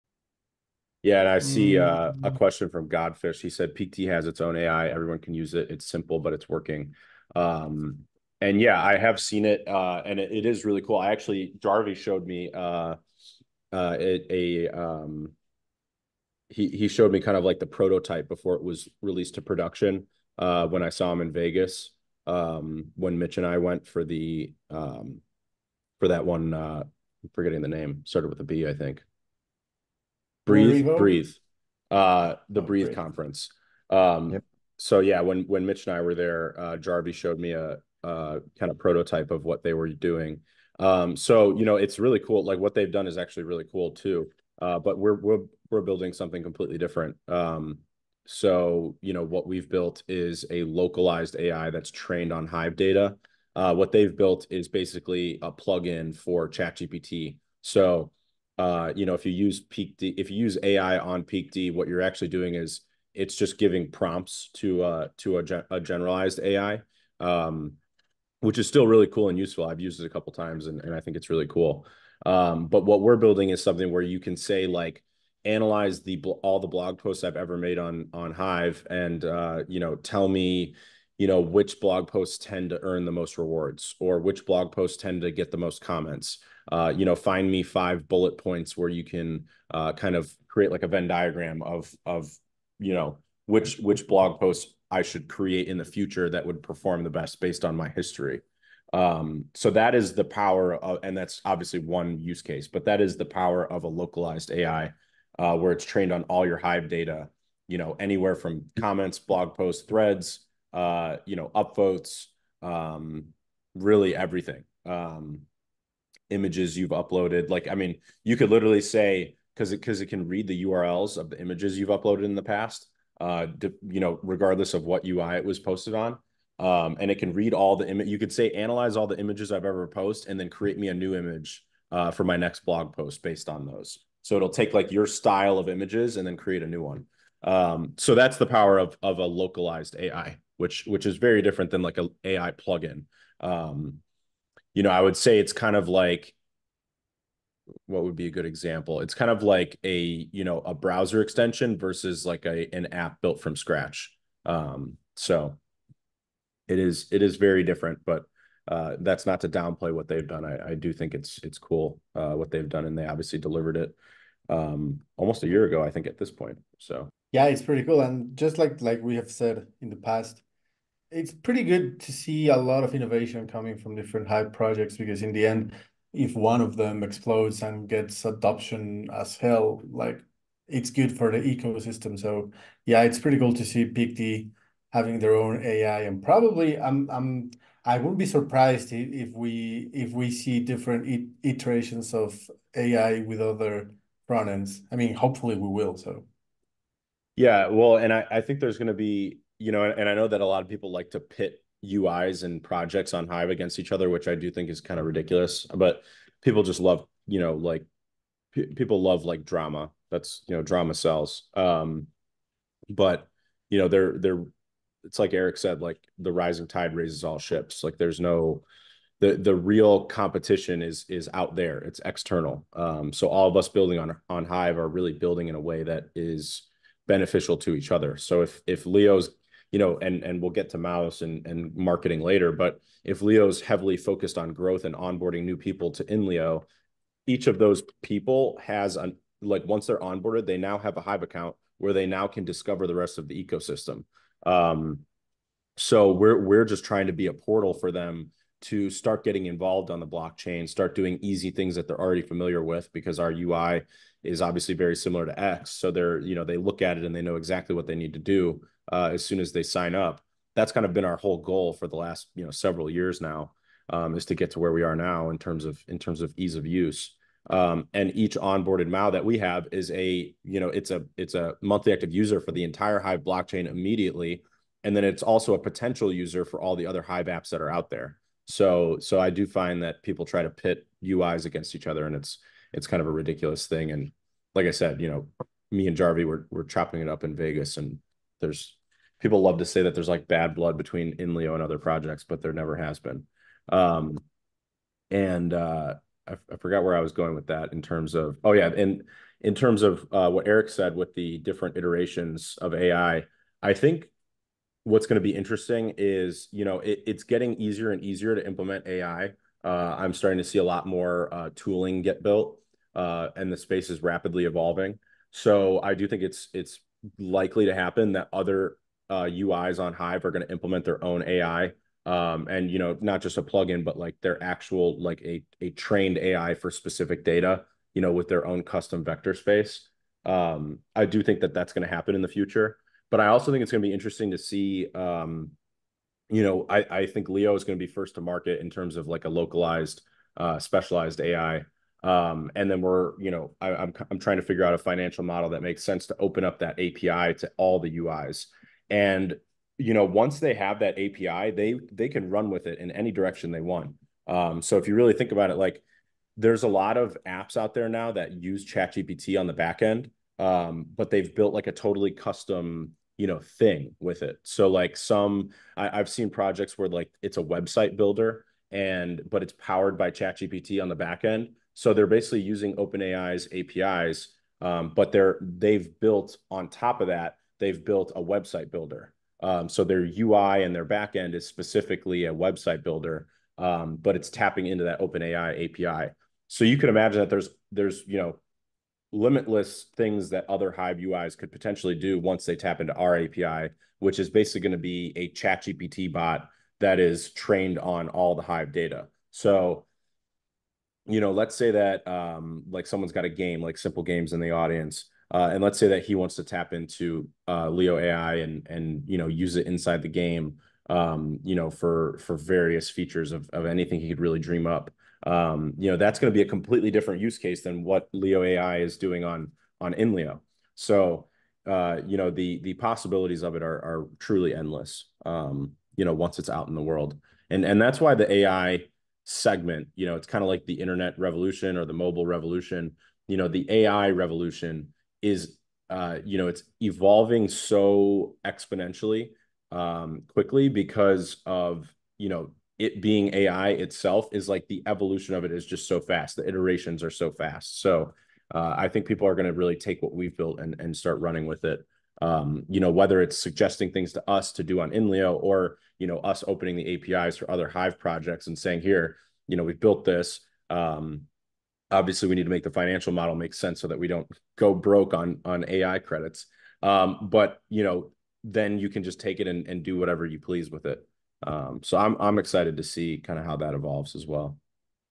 LeoAI: Hive's Creator Experience will be Positively Impacted by the Introduction of AI
Hi everyone! The role of AI in content creation, management, and other areas has been controversial, but now it has become necessary to speed up processes and stay competitive. We won't be left behind, which is why we've created LeoAI. We can already see other UIs incorporating AI, and we expect to see more in the future.

In this clip:

- An AI plug-in and locally trained AI are different; LeoAI is a Hive trained AI
- AI implementation is becoming easier, so we'll see more of it in the future
- LeoAI will have an API available to other UIs
- Innovations coming to all Hive UIs benefit the entire ecosystem

What other UIs like PeakD have done with AI on Hive is impressive, but LeoAI is different. It's more than just a plug-in that generates prompts for specific actions; LeoAI is trained on the Hive blockchain. This enables it to analyze your content and provide tailored suggestions, from checking grammar to identifying patterns in your successful content.

AI implementation is becoming simpler, so we anticipate seeing it applied to more UIs, not just as plug-ins but as AI trained on specific datasets. In fact, LeoAI will offer an API that other UIs can integrate and utilize as they see fit, impulsing the creation of more innovative features.

Every improvement by each UI contributes to the growth of Hive as a whole, benefiting all projects involved. At Inleo, we're committed to continuing our development efforts to elevate Hive to new heights.